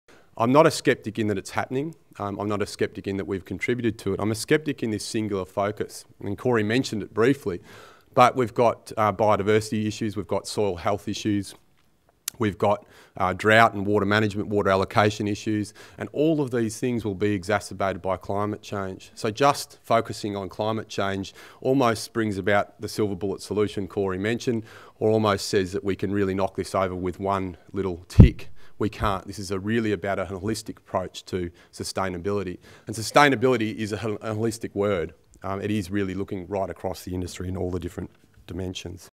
Climate Change Will Impact Sustainable Farming | Can Red Meat Be Green?
Target 100 is an initiative by Australian cattle and sheep farmers to deliver more sustainable cattle and sheep farming by 2020.

Environmentalist, Arron Wood explains that he is not a sceptic that climate change is happening, but he is that climate change is the only focus, as there are other important environmental considerations including biodiversity, water and soil health that will all be exacerbated by climate change. By just focussing on climate change it ignores this and we need to look at a holistic approach to sustainability.